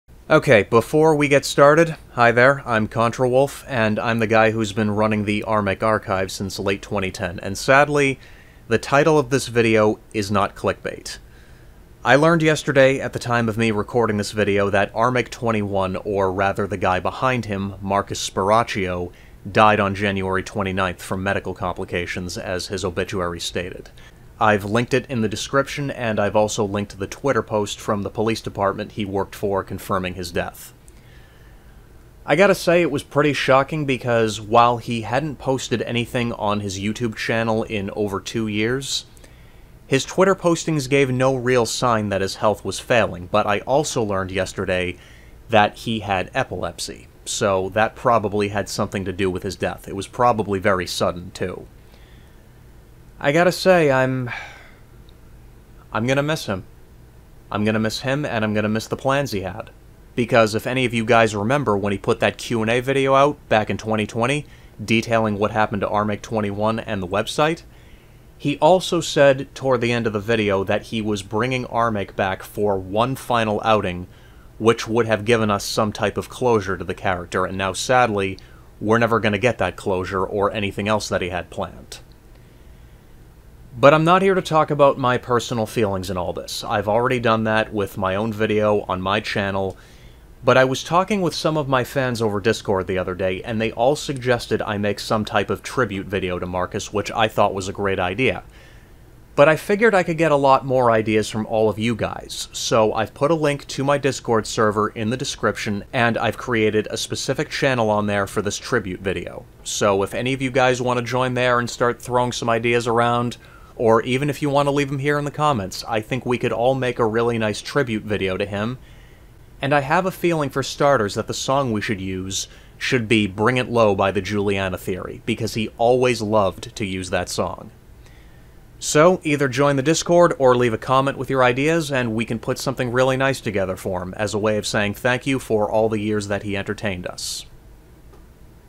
RIP Armake21
I wish I could say this was a clickbait video but I can't, Marcus Sparacio, the guy behind Armake21, has died